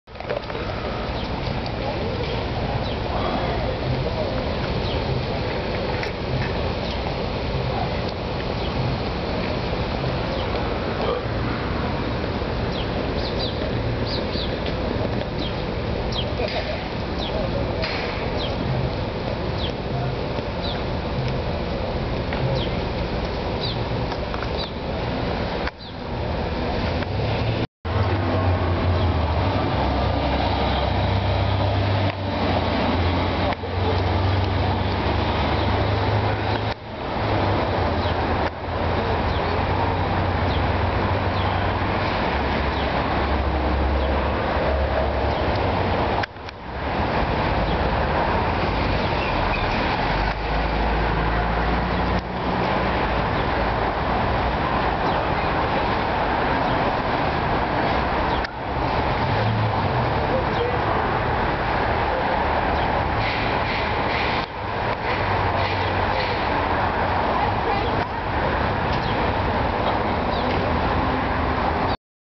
12 April 2009 LACC Ops Personnel in golf cart on Vermont Ave sidewalk
LACC Operations personnel: working hard to impede pedestrians and students alike.